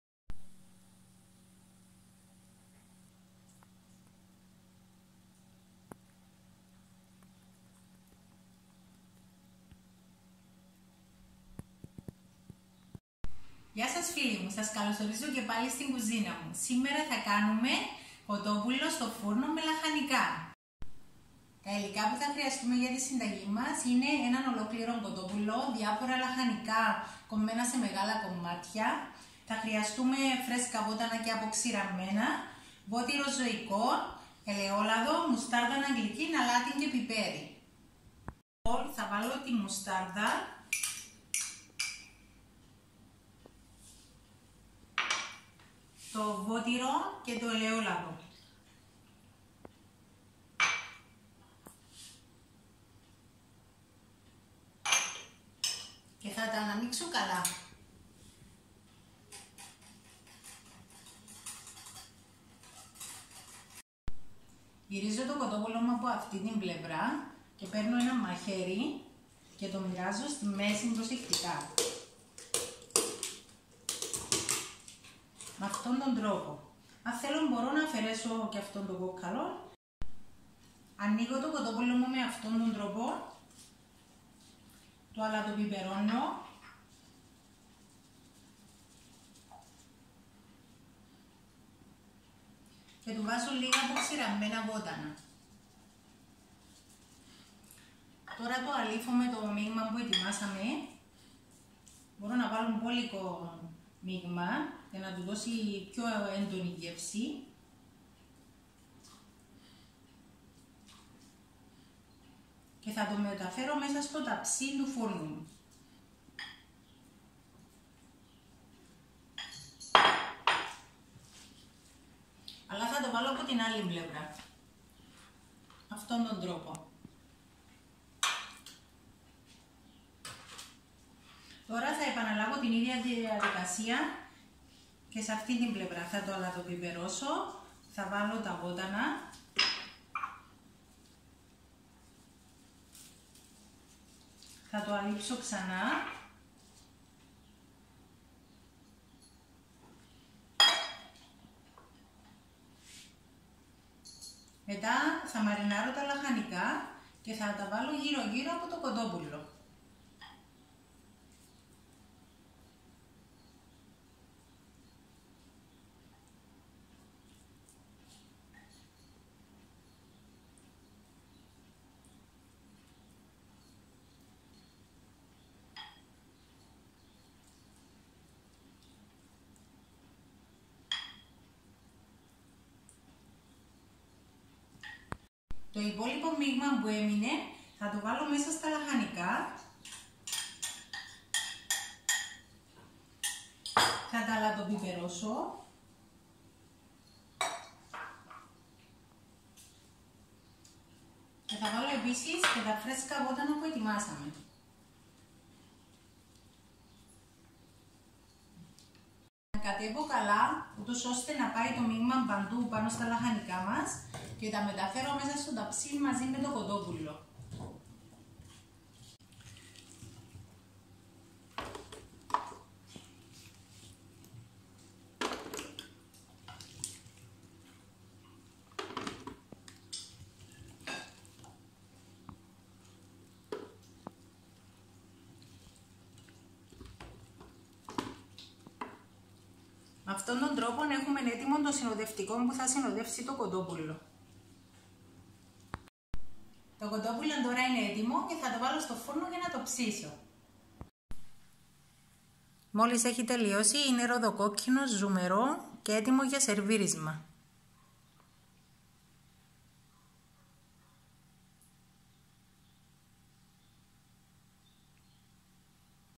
Κοτόπουλο στο φούρνο με Λαχανικά. Πανεύκολο και ζουμερό. Chicken in the oven with vegetables.
Κοτόπουλο με Λαχανικά στο φούρνο.

1 ολόκληρο κοτόπουλο περίπου 2 κιλά.
Διαφορά λαχανικά κομμένα σε μεγάλους κύβους (καρότο κρεμμύδι κολοκιθια, λαχανάκια Βρυξελλών κ. α. )
1 κιλό πατάτες καθαρισμένες κομμένες κύβους.

Μαριναδα:
Μισό ποτήρι ελαιόλαδο.
1 κ. σ μουσταρδα αγγλική.
100 γρ. Βοτύρο ζωικό λιωμένο.

Διαφορά φρέσκα και αποξηραμένα βότανα .
Αλάτι, πιπέρι.(ρίγανη, θυμάρι λασμαρι, βασιλικό )

 Προαιρετικά :

1 σκελιδα σκόρδο.
Ξύσμα ενός λεμονιού.

Οδηγίες :
Ανακατεύω όλα τα υλικά της μαριναδας σε ένα μπολ.
Ετοιμάζω τα λαχανικά και τις πατάτες.
Χωρίζω το κοτόπουλο με ένα μαχαίρι η ψαλίδι, απο την ραχοκοκαλιά μεχρι την άκρη. Χωρίς να το μοιράσω στα δύο.
Το αλατοπιπερωνω, το αλείφω καλά με το μίγμα και απο τις δύο πλευρές.
Το μεταφέρω στο ταψί με το ανοιγμένο μέρος προςτα κάτω.του βάζω επιπρόσθετα λιγο αλάτι, φρέσκα βότανα και λιγο 3-4 κ. σ. Ελαιόλαδο.
Με το υπόλοιπο μίγμα και λίγο ακόμα ελαιόλαδο επιπρόσθετα μαρινάρουμε τα λαχανικά και τις πατάτες και τα βάζω γύρο γύρω απο το κοτόπουλο.
Το ψήνω στο φούρνο χωρίς σκέπασμα
και χωρίς επιπρόσθετα υγρά για περίπου δύο ώρες στους 180 βαθμούς.